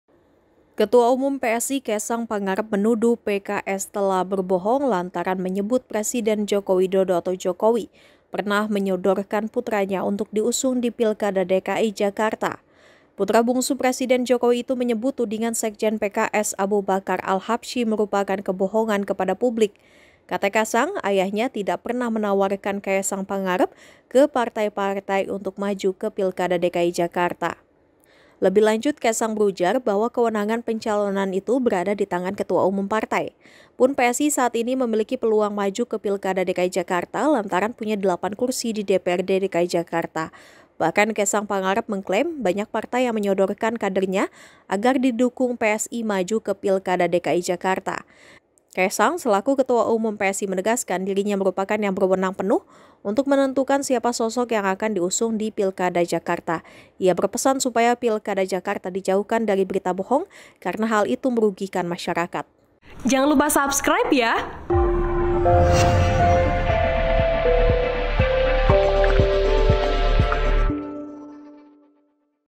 Kaesang Pangarep Sebut PKS Bohong Soal Jokowi Cawe-cawe Pilkada
Ketua Umum PSI Kaesang Pangarep menuduh PKS telah berbohong lantaran menyebut Presiden Joko Widodo (Jokowi) pernah menyodorkan putranya untuk diusung di Pilkada DKI Jakarta.

Putra bungsu Presiden Jokowi itu menyebut tudingan Sekjen PKS Aboe Bakar Al Habsyi merupakan kebohongan kepada publik.

Kata Kaesang, ayahnya tidak pernah menawarkan Kaesang Pangarep ke partai-partai untuk maju ke Pilkada DKI Jakarta.

"Pak Jokowi tidak pernah menawarkan nama saya ke partai-partai. Silakan cek, atau sebut partai mana yang pernah ditawari Pak Jokowi."

sumber          :  Kompas.com
Penulis         :  Desy Selviany
VO              :  Desy Selviany
Editor Video    :  B.Agus Susanto